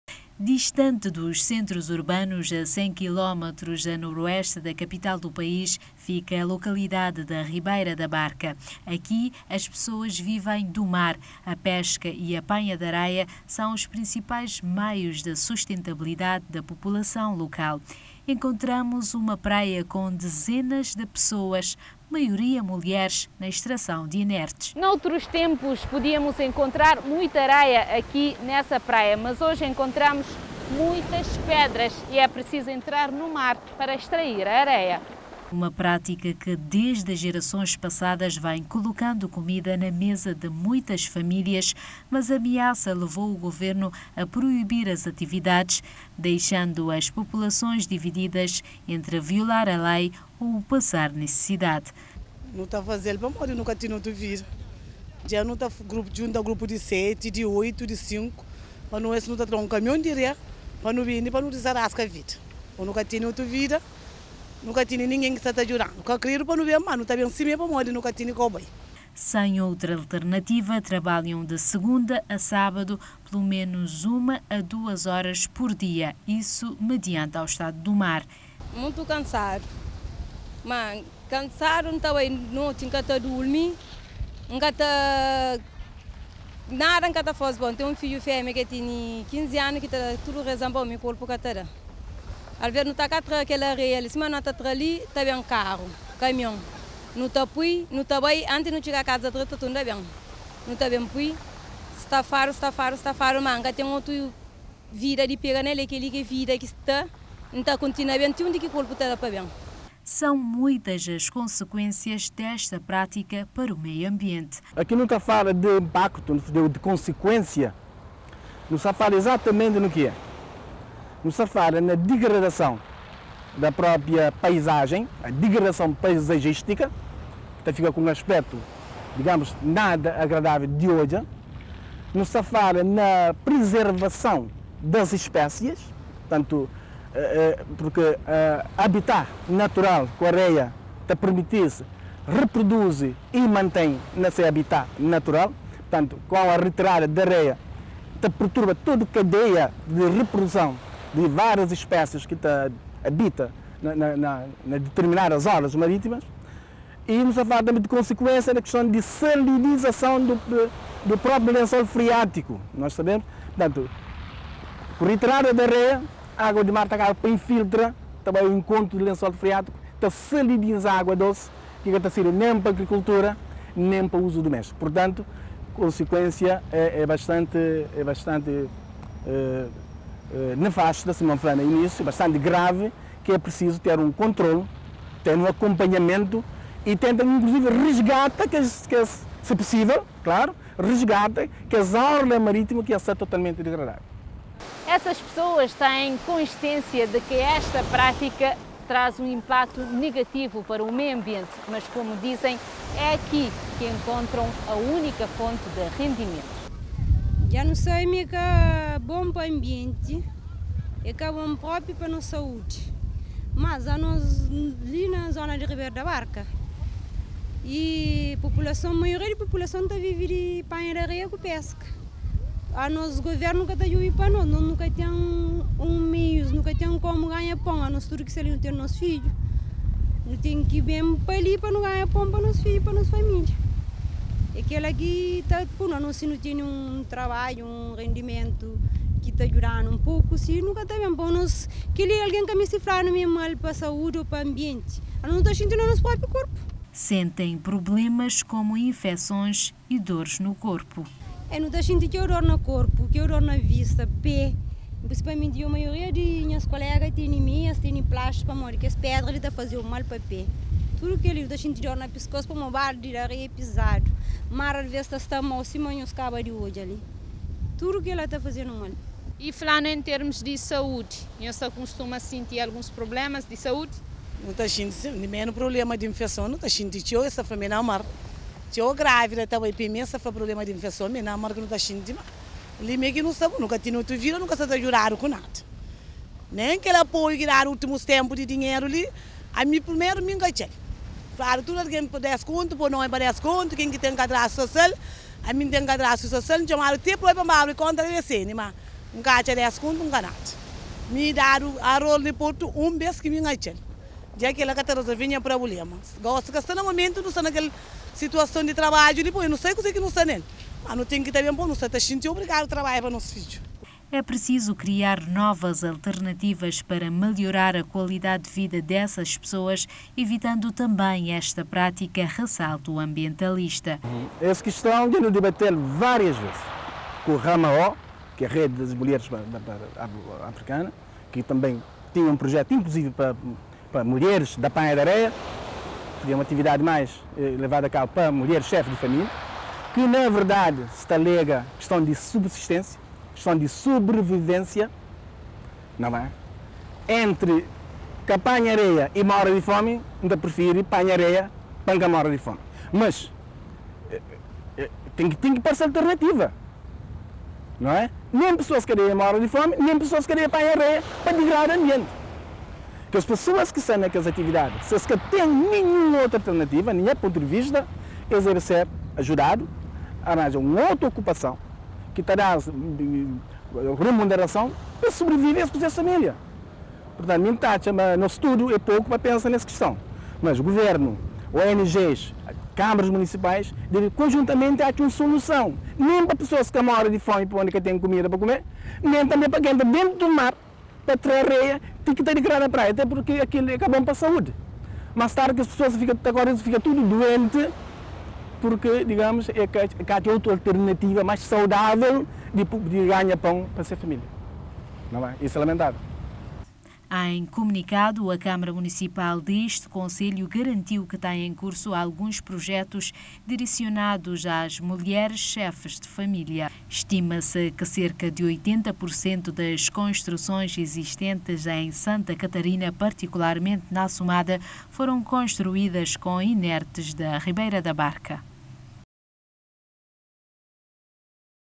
RIBEIRA BARCA: APANHA DESENFREADA DA AREIA NAS ORLAS MARíTIMAS
Boa parte da população de Ribeira Barca sobrevive da extração de areia, meio de sobrevivência que tem afetado a natureza naquela zona.
👇 Acompanhe também👇📌